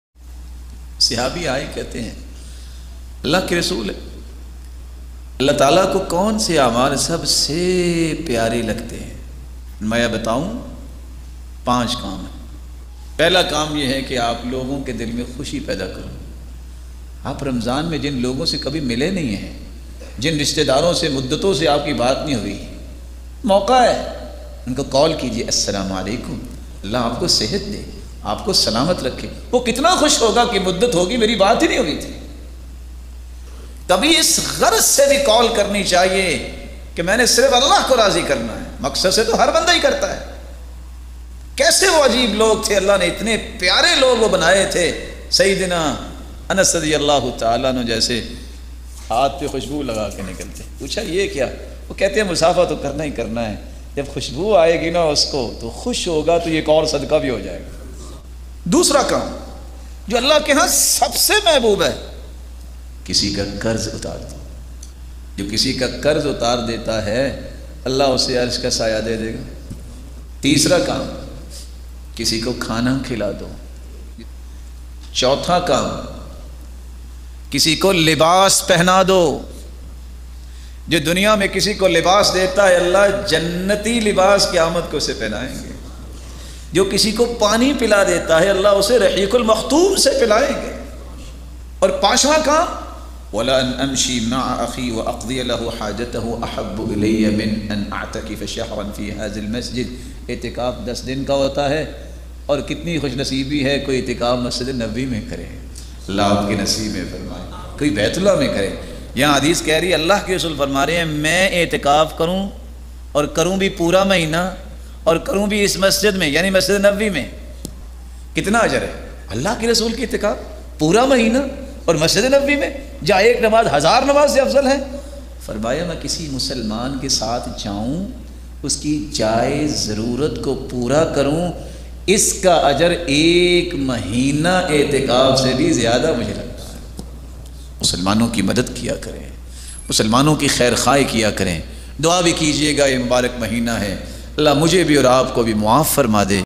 Allah Ta'ala Ko Mahboob 5 Amal | Sohaib Ahmed Meer Muhammadi New Bayan 2022
Speeker : Qari Sohaib Ahmed Meer Muhammadi
Chief Executive : Quraan College and Islamic Training Institutea, Phool Nager
District Kasur, Pakistan.